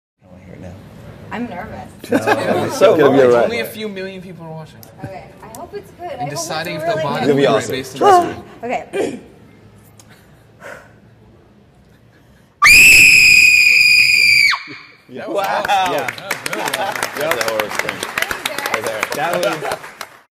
Evil Dead's Jane Levy - The Perfect Horror Movie Scream
Evil Dead's Jane Levy shows us how it's done.